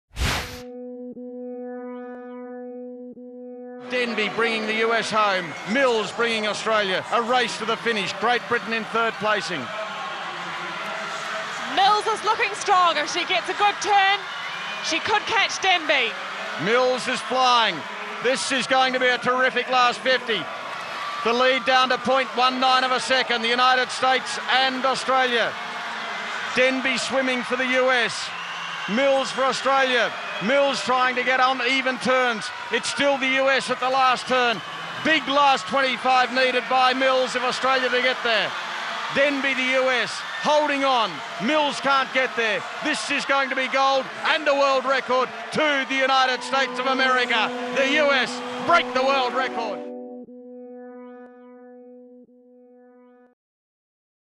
The US Women Set World Record in 4x100m Medley Relay
Just as the American men did a few days ago, the US Women win gold in the 4x100 Medley Relay and set a world record as well.  Margaret Hoelzer, Jessica Hardy, Rachel Komisarz and Kara Denby not only win for the first time, but set a new record at three minutes 51.36 seconds at the 9th FINA World Swimming Championship in Manchester, England. (WCSN, WCSN.com)